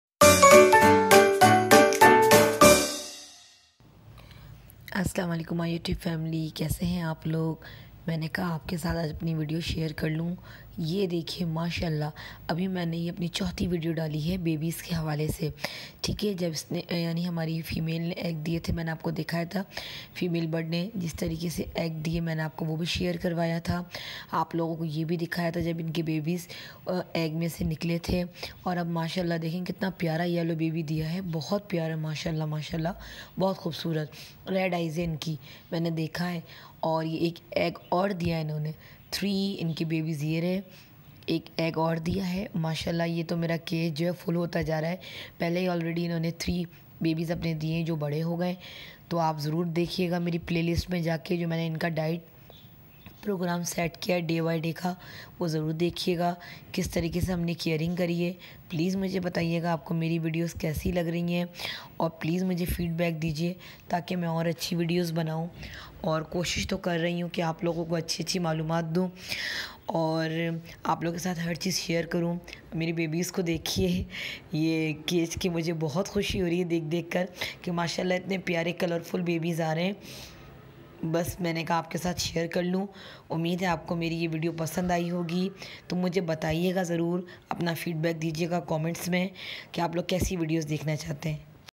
Budgies Baby Baray ho rahein hai 🤗🤗🐦🐦||Budgies Baby Growth Fast
Budgies Baby Baray ho rahein hai 🤗🤗🐦🐦||Budgies Baby Growth Fast||‎@PetsAndMom
Hmare budgies baby  baray ho Rhein hai ma Sha ALLAH 🤲 bht cute h h 🤗